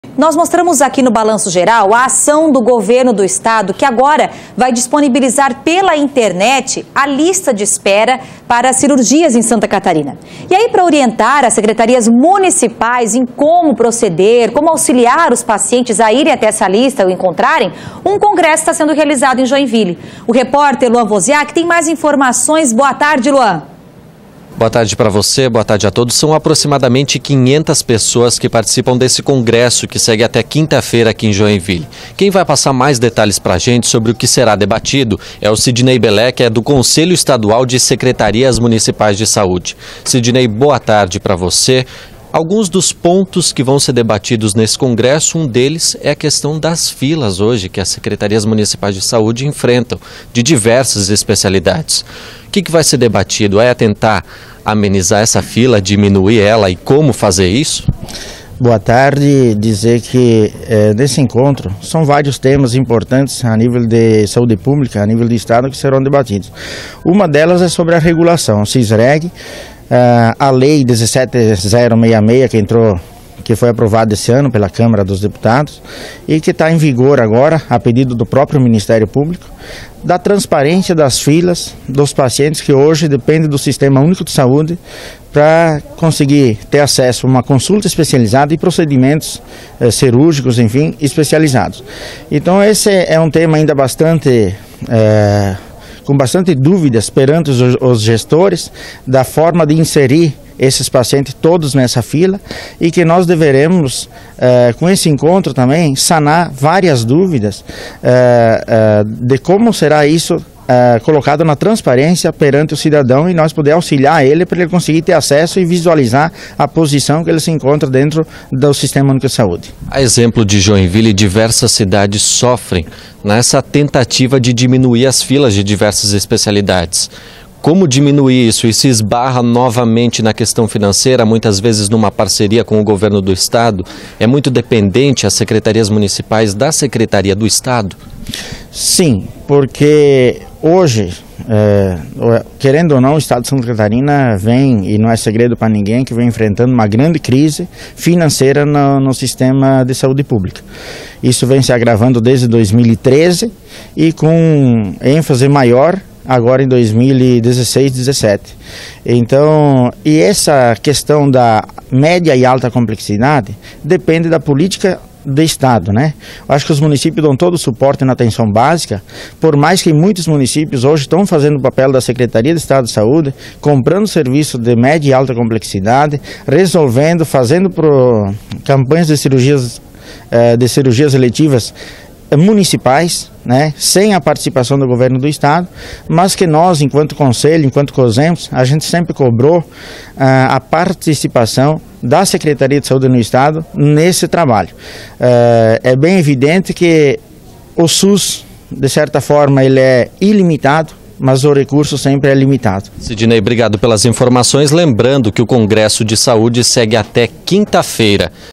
Congresso debate transparência na lista de espera para cirurgias em Santa Catarina
Uma ação do governo do estado agora vai disponibilizar pela internet a lista de espera para cirurgias em Santa Catarina. Para orientar as Secretarias Municipais em como proceder e auxiliar pacientes um congresso é realizado em Joinville.